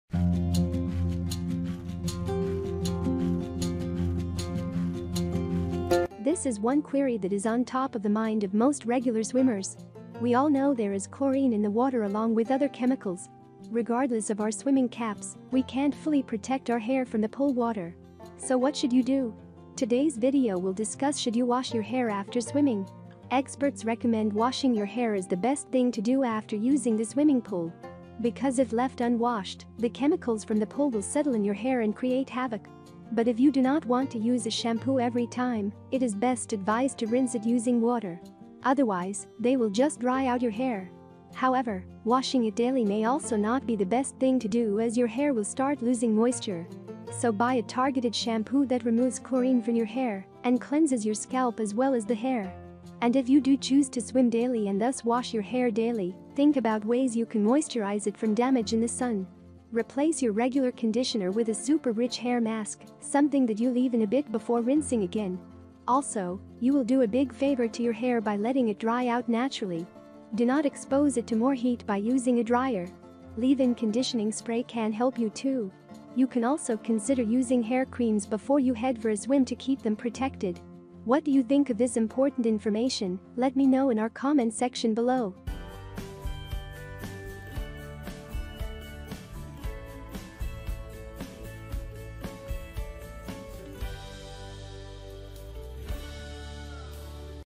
Should You Wash Your Hair After Swimming
This is one query that is on top of the mind of most regular swimmers. We all know there is chlorine in the water along with other chemicals.
Regardless of our swimming caps, we can't fully protect our hair from the pool water. So what should you do?

Today's video will discuss Should you wash your hair after swimming.

Experts recommend washing your hair is the best thing to do after using the swimming pool. Because if left unwashed, the chemicals from the pool will settle in your hair and create havoc. But if you do not want to use a shampoo every time, it is best advised to rinse it using water. Otherwise, they will just dry out your hair.

However, washing it daily may also not be the best thing to do as your hair will start losing moisture. So buy a targeted shampoo that removes chlorine from your hair and cleanses your scalp as well as the hair.
And if you do choose to swim daily and thus wash your hair daily, think about ways you can moisturize it from damage in the sun. Replace your regular conditioner with a super-rich hair mask - something that you leave in a bit before rinsing again.

Also, you will do a big favor to your hair by letting it dry out naturally. Do not expose it to more heat by using a dryer. Leave-in conditioning spray can help you too.
You can also consider using hair creams before you head for a swim to keep them protected.

What do you think of this important information, Let me know in our comment section below?